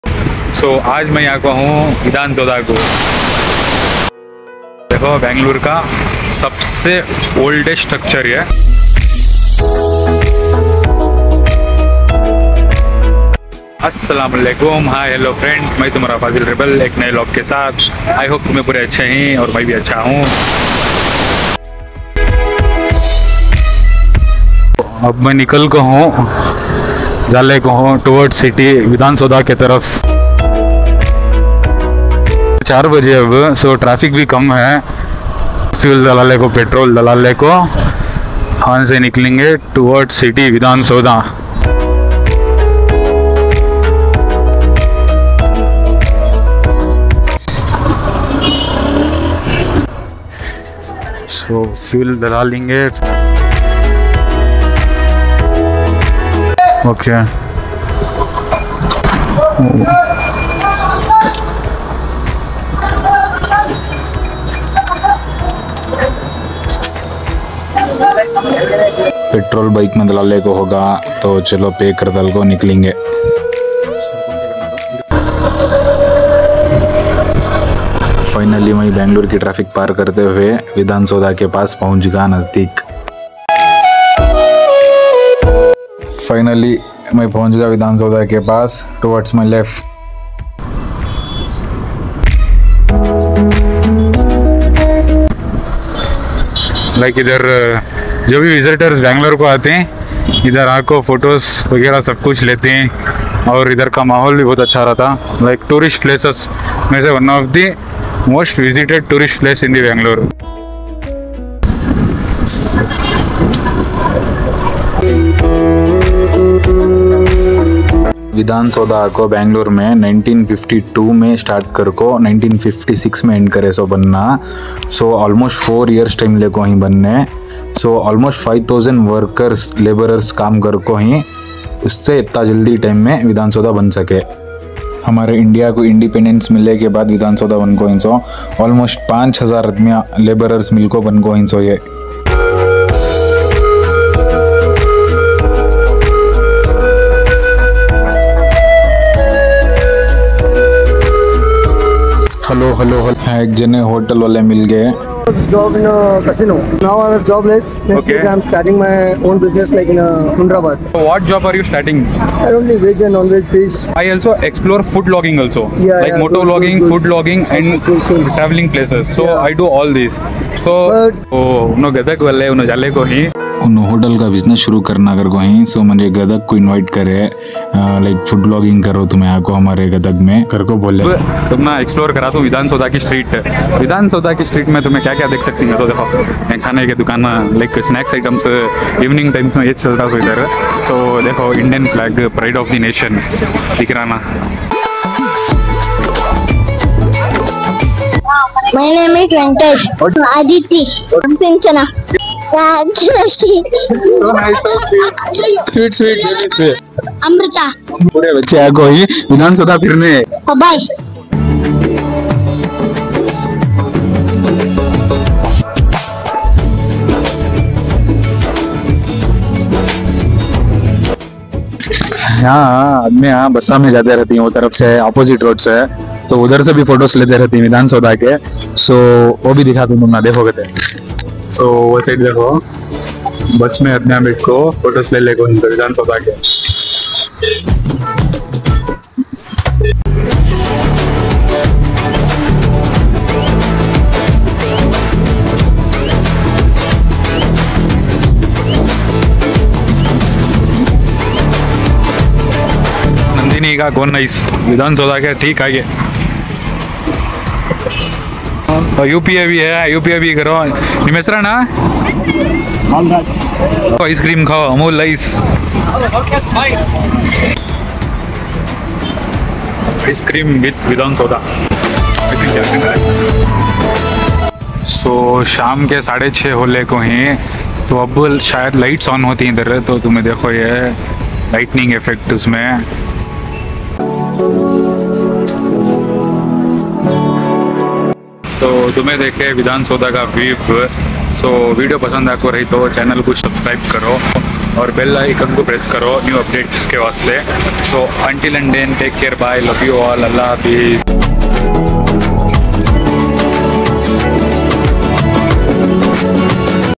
Vidhana soudha | Bangalore Vidhana Soudha Vlog | Bengaluru
Vidhana Soudha Bangalore vlog Tourist place in Namma Bengaluru.The Vidhana Soudha, a well-known landmark of Bangalore, was constructed in 1954.

Location📍

Fazil Shaikh

# Fazil Rebel

Fazil Shaikh
From - Bangalore Karnataka (India)